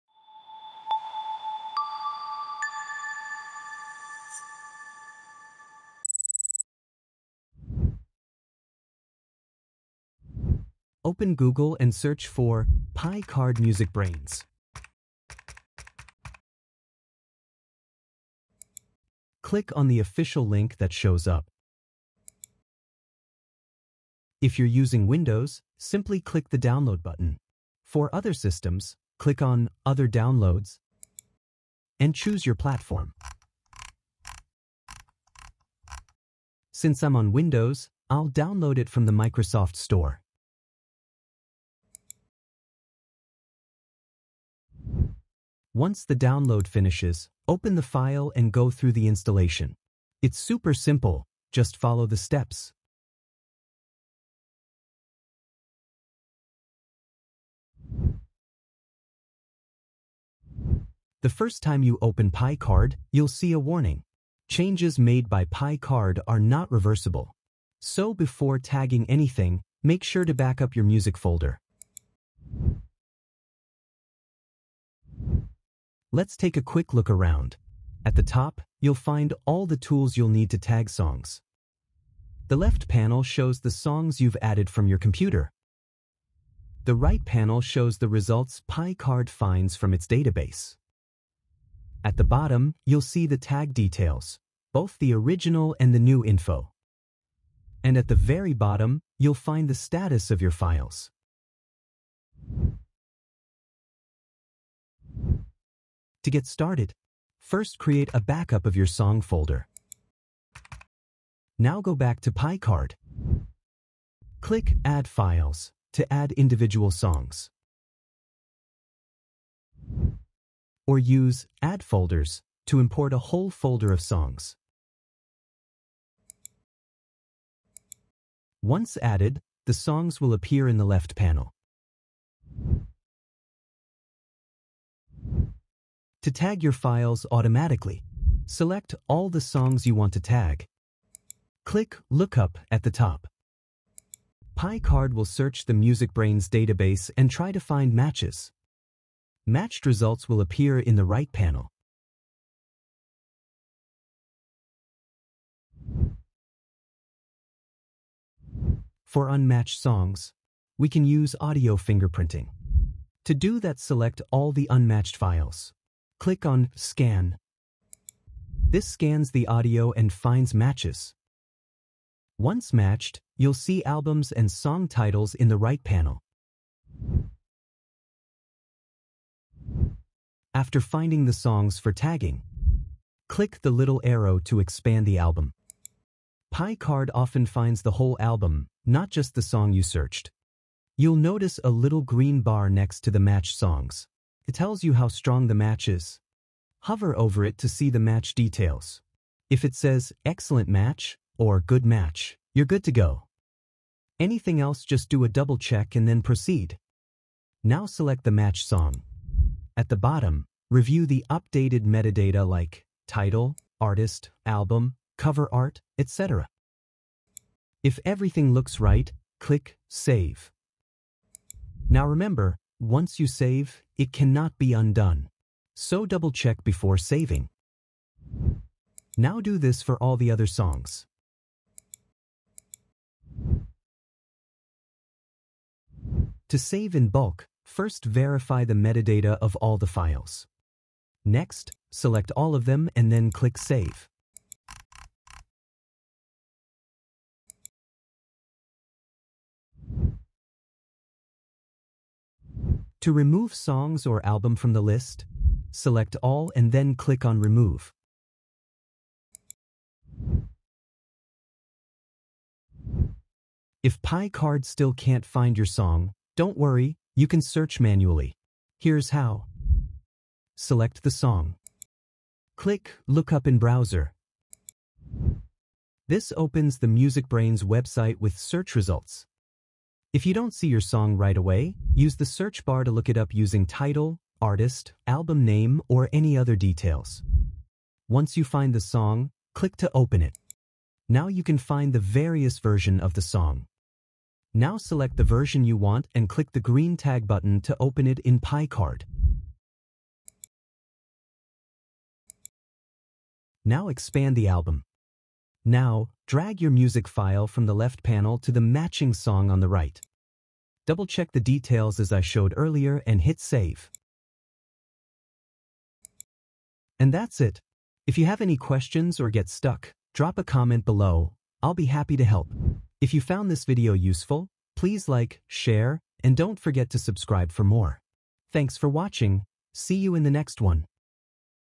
MusicBrainz Picard for Beginners | MP3 audio Tag Manager | 2025
In this video, I walk you through how to use MusicBrainz Picard—a free, cross-platform tagging tool—to automatically search for and tag your MP3 and other music files with accurate metadata. Powered by the extensive MusicBrainz database, Picard helps organise your music library with precision and ease.

0:00 - Intro
0:10 - Downloading & Installing
0:58 - Creating Backup
1:12 - Picard Musicbrainz Software Overview
1:43 - Getting Started with Picard Musicbrainz
2:12 - Looup Feature
2:35 - Scan or Audio Fingerprinting Feature
2:57 - Verifying the Metadata or Information
3:54 - Saving tags or Metadata in bulk using Picard Musicbrainz
4:10 - Removing from the list
4:20 - Manual search using the Lookup in Browser feature

1.
⏩ Other tutorials to check out